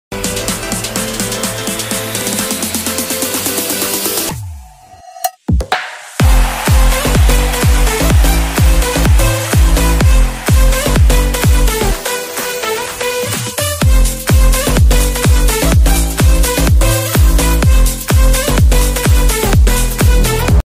Delivery!
A total of two days, a total of 7 cars of goods, all completed shipment, delivery!